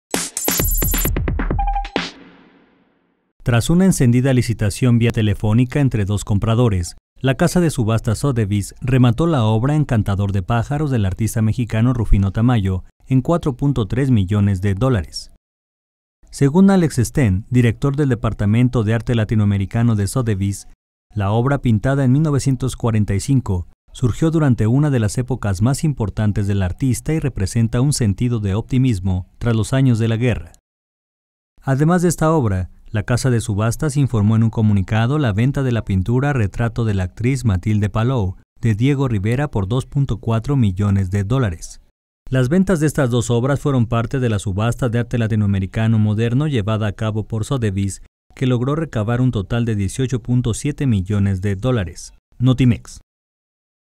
Venden obra de Tamayo en 4.3 mmd en subasta
La casa de subastas Sotheby´s remató la obra "Encantador de pájaros" del artista mexicano Rufino Tamayo en 4.3 millones de dólares